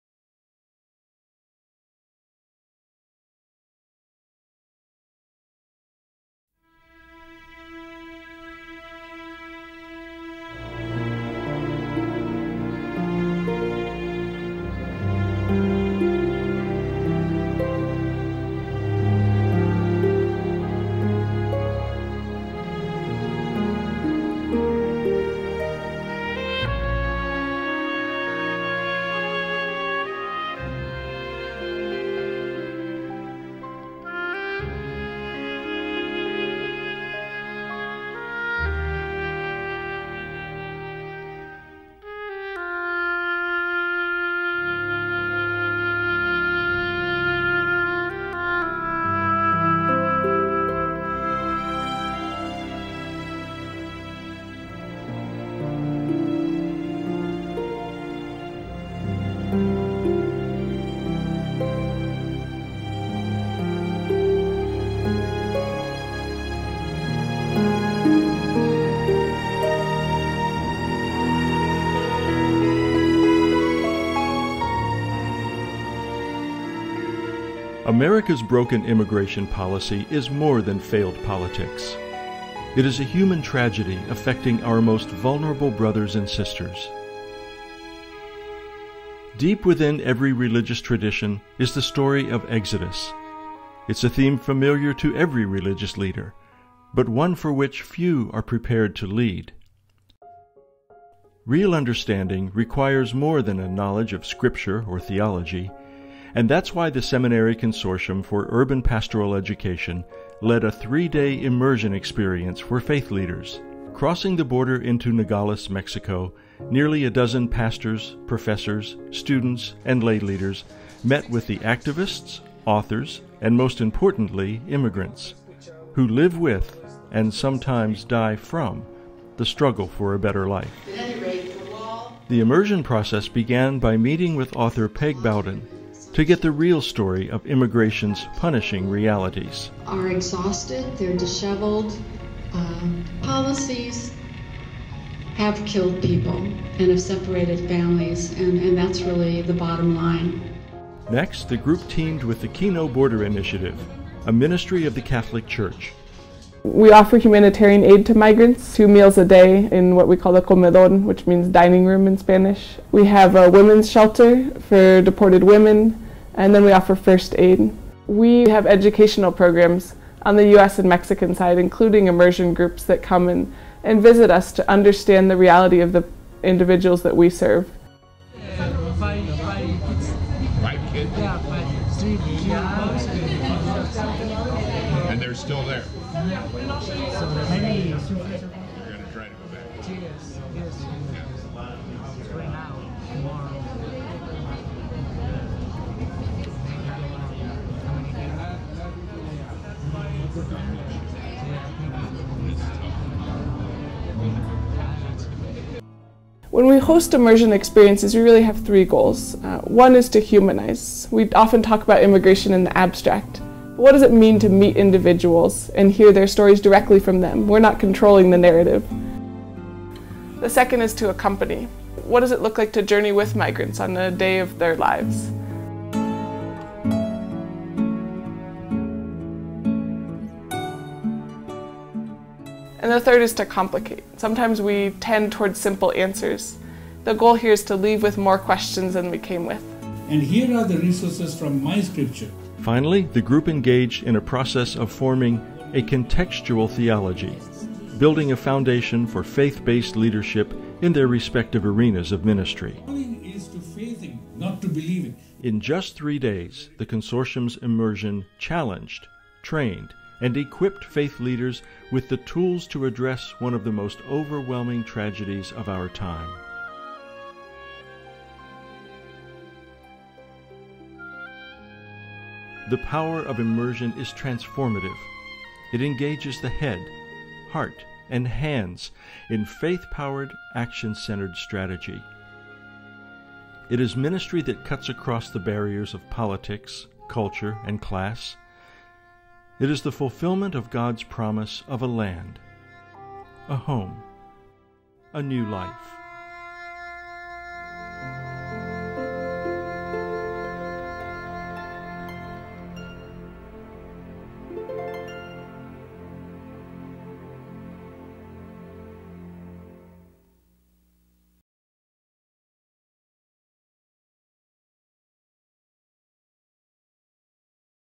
At the Border of Hope: An Immersion Experience
Filmed in Nogales, Mexico, see how SCUPE's Immersion program challenges, trains, and equips faith leaders with the tools to address one the most overwhelming tragedies of our time - the plight of immigrants seeking a better life in the US.

Immigrants face more than a wall - they struggle with horrific danger, psychological pain, and fear of separation fed by a broken political system threatening to break apart families and destroy lives.  SCUPE's Immersion program is the first step in building a contextual theology, the foundation for action-centered faith-based leadership - both at the border and in the corridors of power.